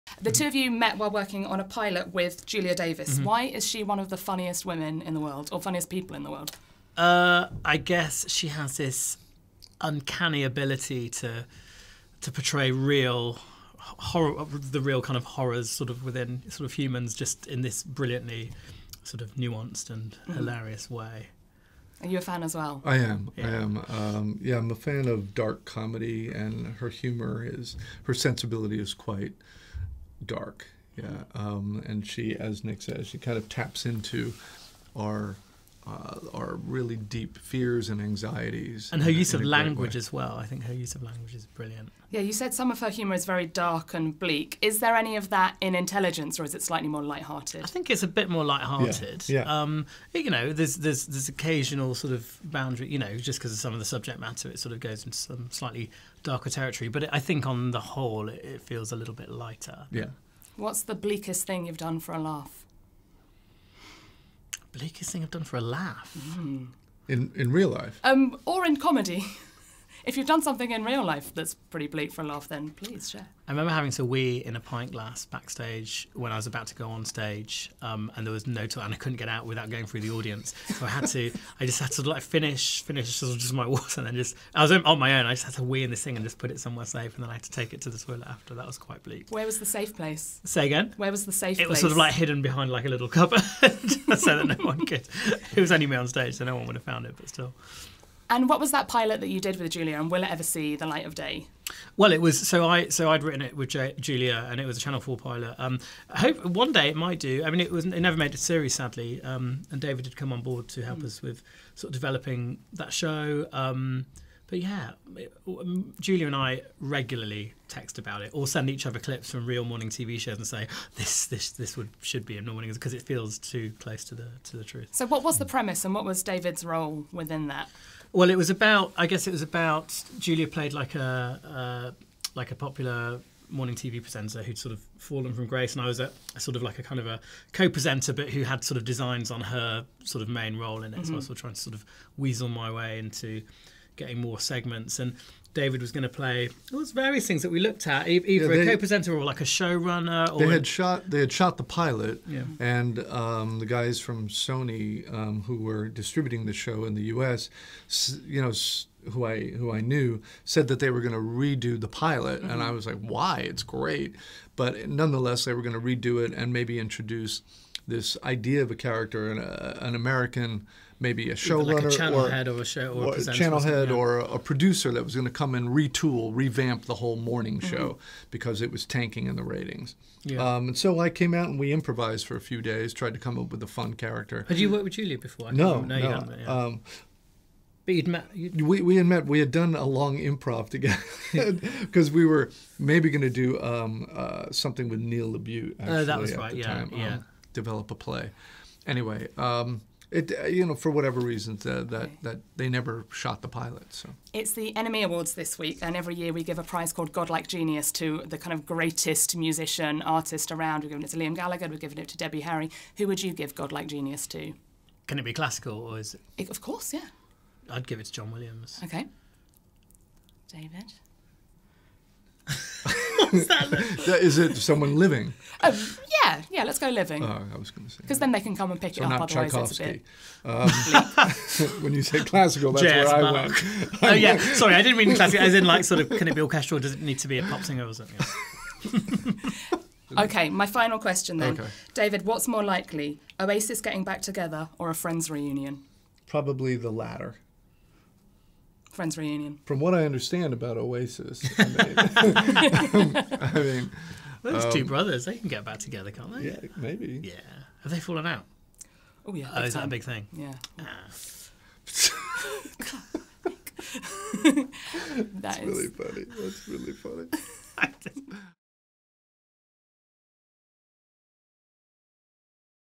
David Schwimmer & Nick Mohammed: 'Friends' reunion or Oasis getting back together?
What's more likely – Oasis getting back together or a 'Friends' reunion? The cast of new Sky sitcom 'Intelligence' spills the beans